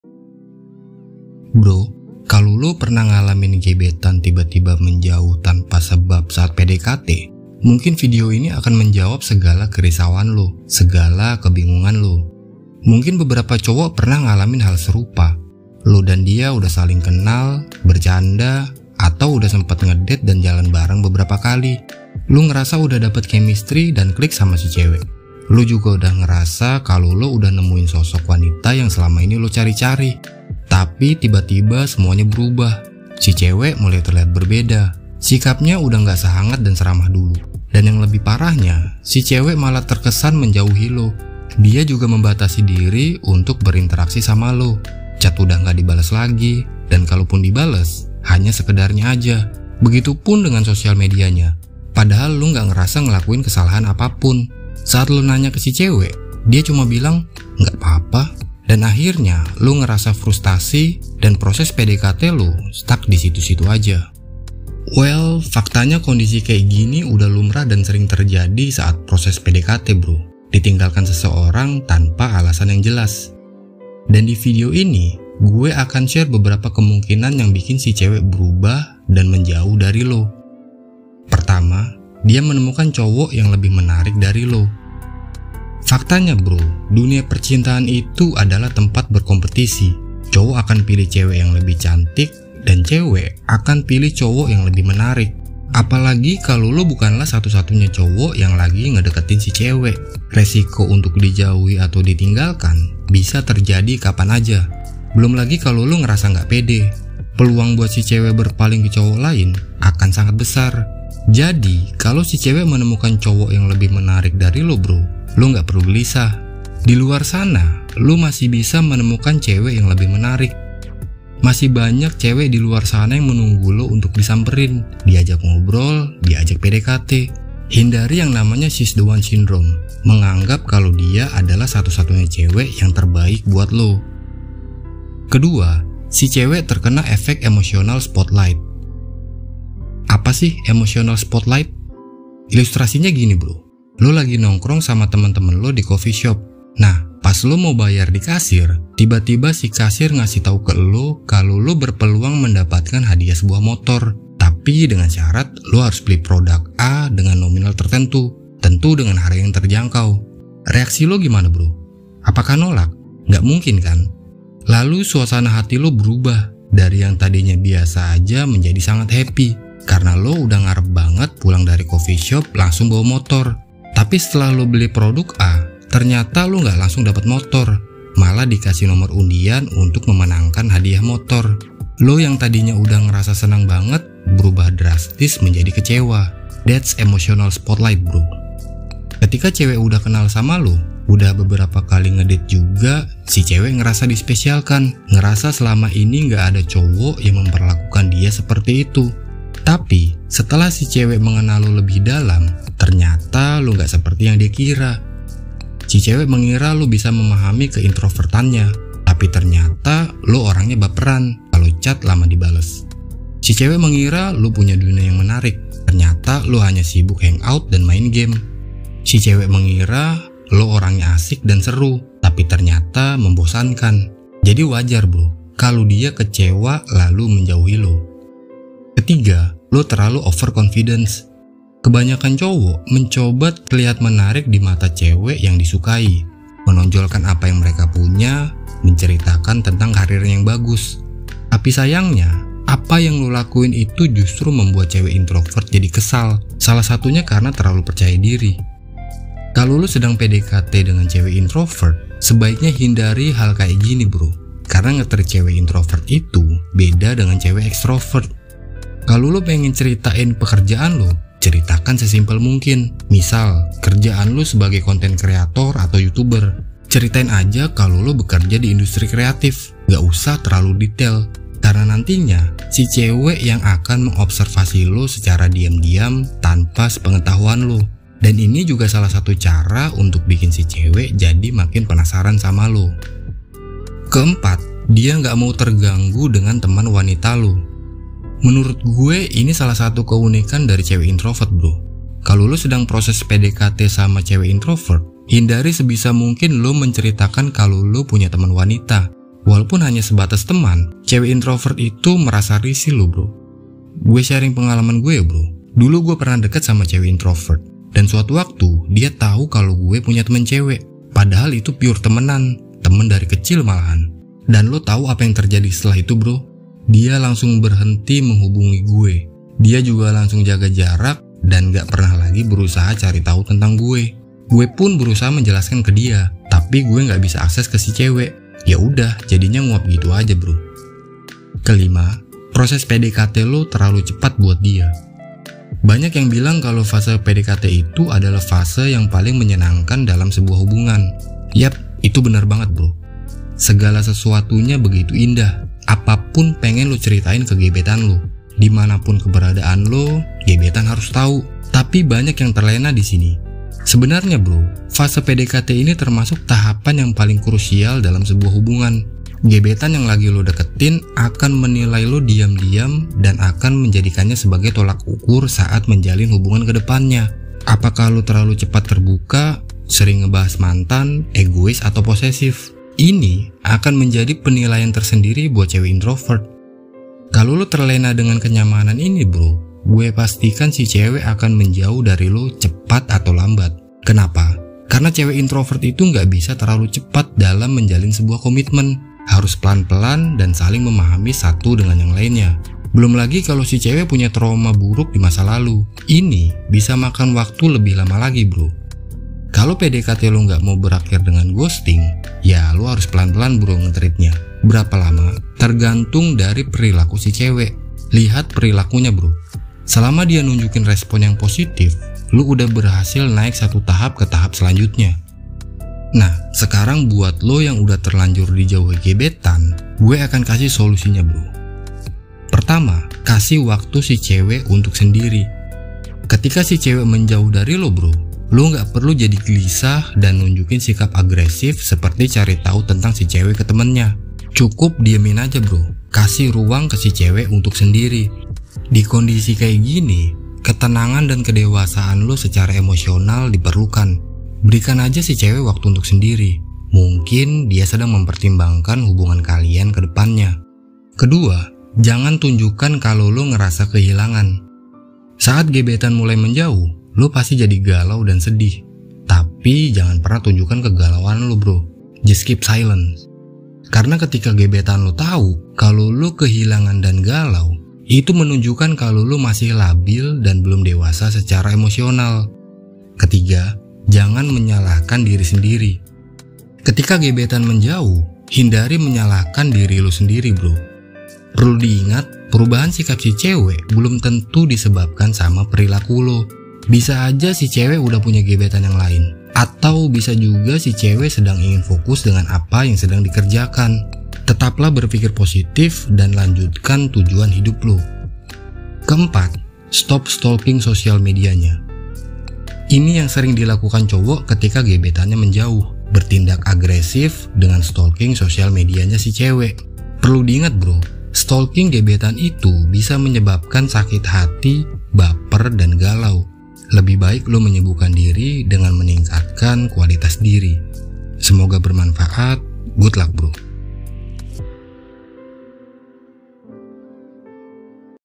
Jangan Lakukan Ini - 5 Alasan CEWEK INTROVERT menjauh Saat PDKT
Ada banyak alasan wanita introvert menjauhi pria saat PDKT. Hal ini bisa saja disebabkan oleh banyak faktor. Bisa karena faktor internal maupun faktor eksternal.
Kalau lo sedang mengalami gebetan tiba tiba menjauh tanpa sebab saat PDKT, video ini akan menjawab semua kebingungan lo bro.

There are many reasons why introverted women shy away from men when approaching them. This could be caused by many factors. It could be due to internal factors or external factors.
If you are experiencing a crush who suddenly walks away for no reason when you approach her, this video will answer all your confusion, bro.